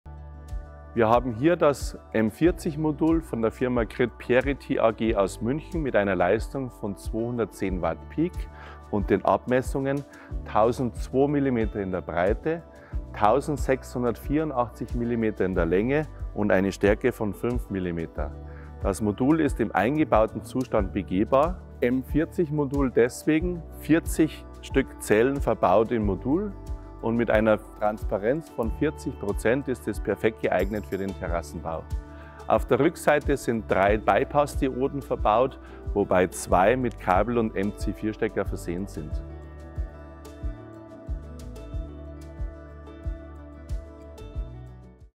GridParity 210 Wp, M40 Photovoltaik Modul, Doppelglas
Detaillierte Beschreibung unseres Doppelglas-Moduls M40 (1684mmx1002mmx5mm) des Herstellers GridParity.